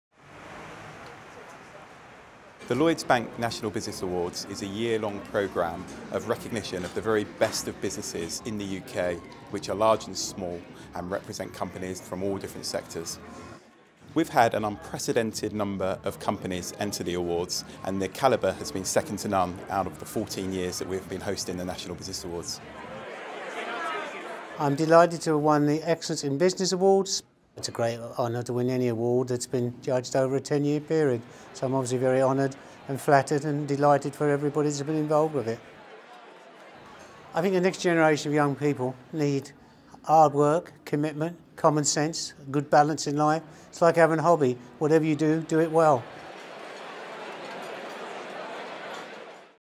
Tony Pidgley CBE wins The Lloyds Bank Excellence in Business Award
Tony left school at 15 to start up his own business in the haulage and plant hire sector. At 19 he sold his business to Crest Homes and became a director before leaving to form Berkeley  Homes which over the next ten years enjoyed considerable growth before it eventually floated on the stock exchange in 1985 as Berkeley Group PLC.
•         The judges said of him that: “He has an uncanny ability to call markets, foresee recessions, as well as put his personal capital at risk when neccessary. His drive and commercial flair has also turned Berkeley into one of Britain’s best residential developers with a reputation for outstanding commercial performance and which has been ranked as the UK’s most sustainable major housebuilder for each of the last eight years.”
•         His expertise has been used to advise the Mayor on the Outer London Commission and the Government disposal of public sector land.
•         He is also a trustee of Open City London, a charity that champions architecture, education as well as being the President of the London Chamber of commerce and industry. And then to cap off a phenomenal story Berkeley Group this year made it into the FTSE 100.
•         The event was held on Tuesday 10th November at the Grosvenor House Hotel Park Lane, London W1